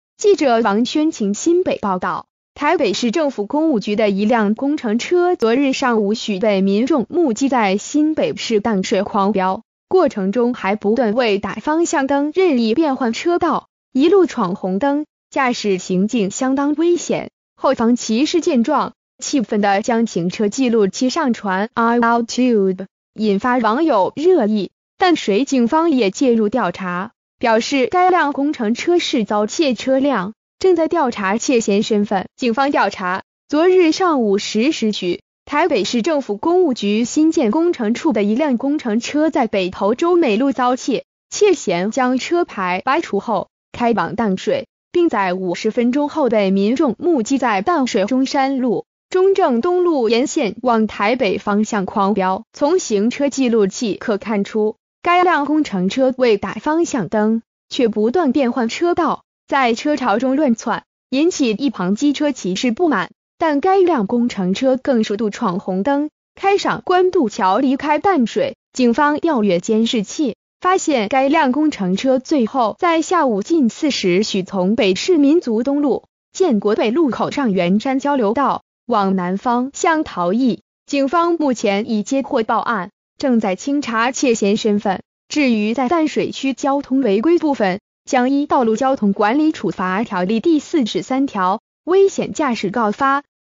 北市工務車淡水危險狂飆 警查出遭竊正追賊
〔記者王宣晴／新北報導〕台北市政府工務局的一輛工程車昨日上午許被民眾目擊在新北市淡水狂飆，過程中還不斷未打方向燈任意變換車道，一路闖紅燈，駕駛行徑相當危險，後方騎士見狀，氣憤地將行車記錄器上傳youtube，引發網友熱議，淡水警方也介入調查，表示該輛工程車是遭竊車輛，正在調查竊嫌身份。

發表評論評論如下。